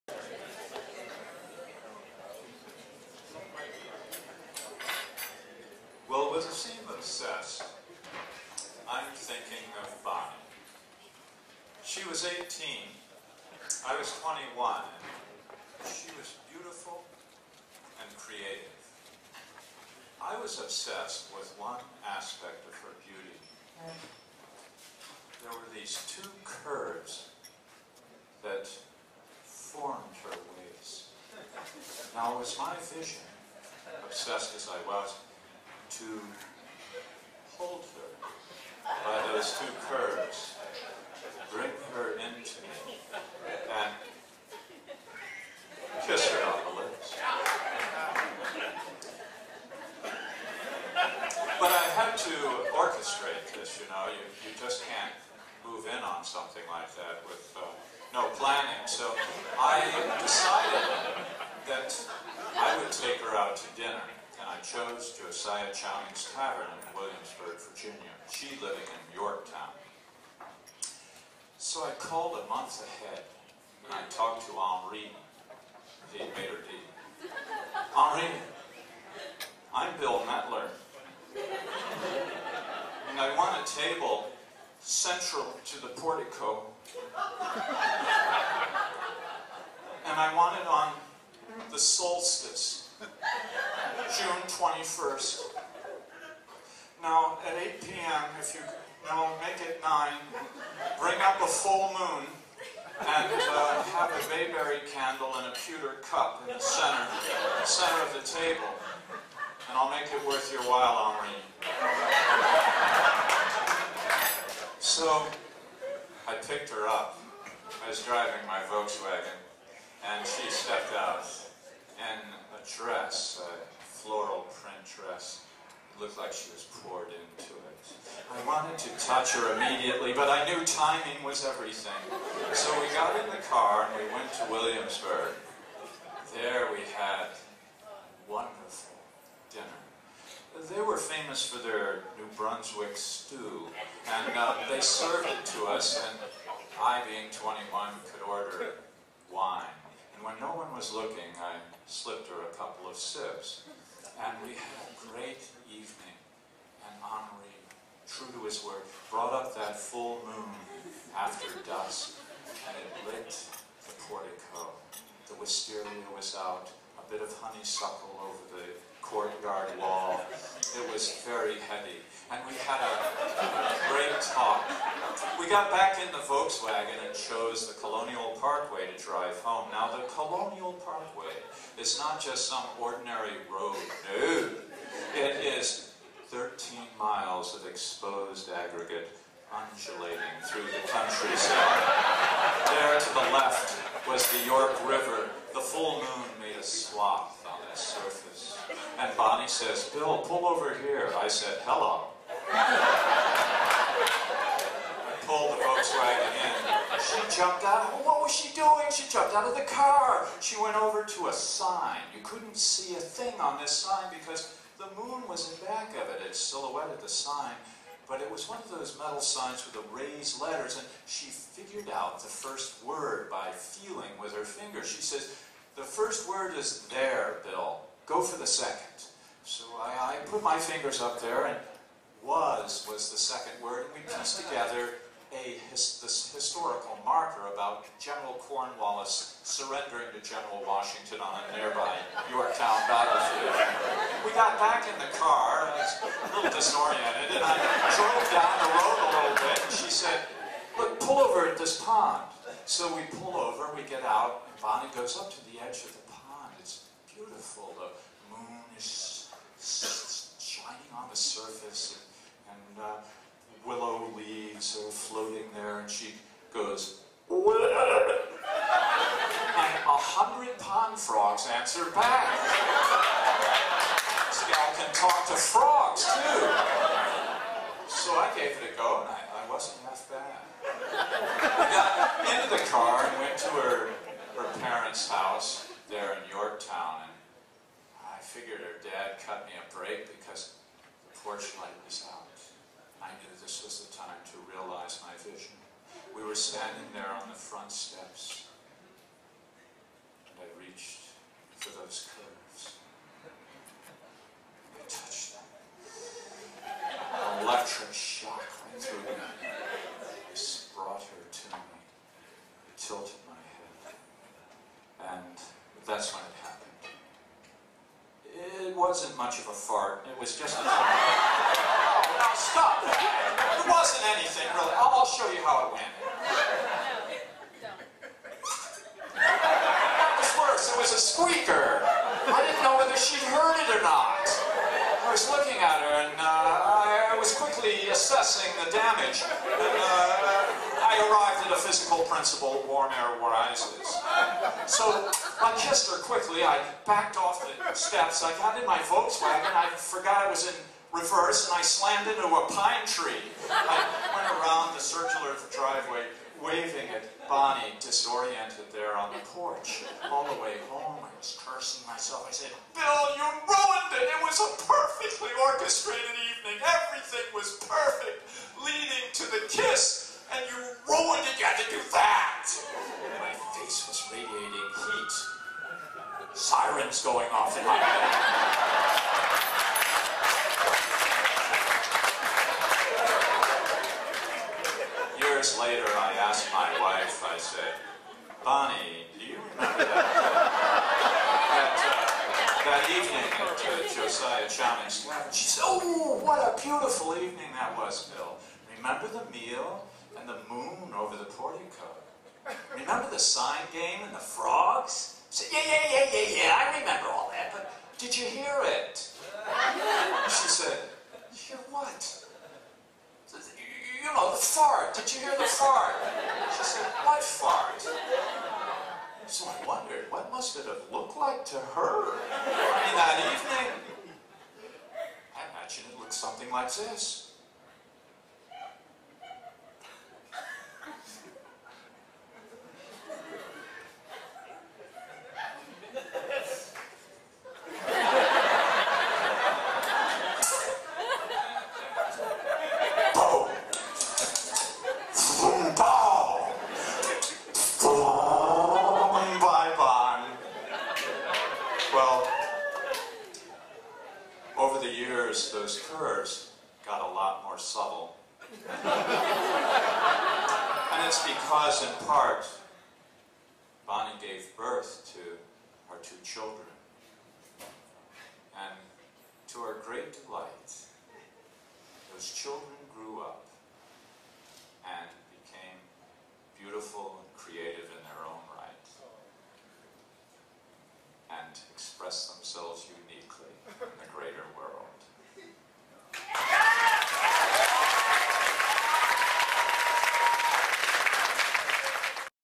Bill, Winning Storyteller and Audience Favorite - Obsessed, First Person Arts StorySlam
Monday, August 13th, 2012
World Cafe Live, 3025 Walnut Street
Theme: Obsessed